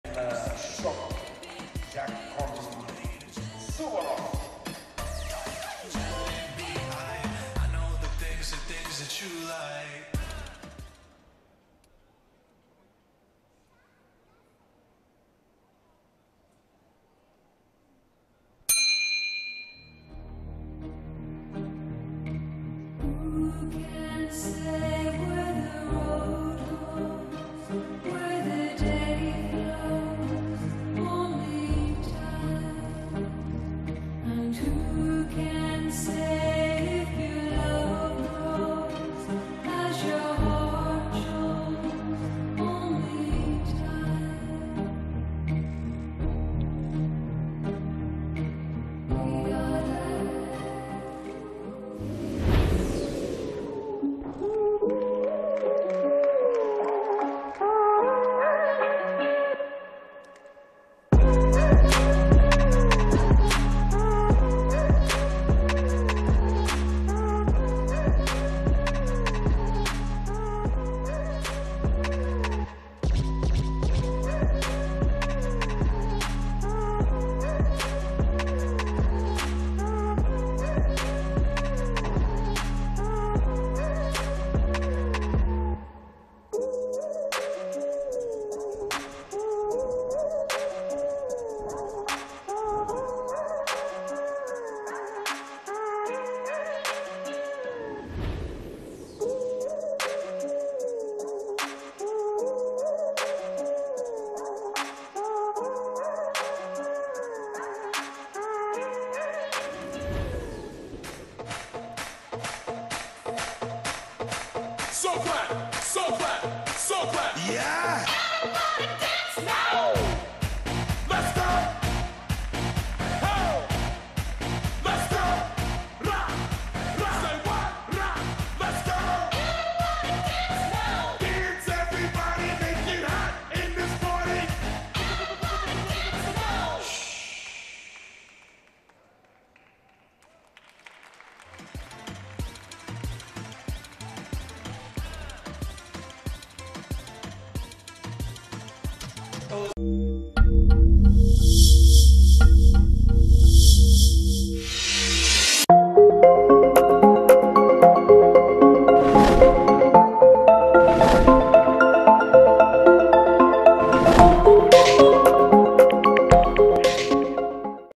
Uzbekistan (UZB) - 2022 Acrobatic Worlds, Baku (AZE) - Dynamic Qualification Men's Group
FIG Official - 28th Acrobatic Gymnastics World Championships - Baku (AZE), March 10-13, 2022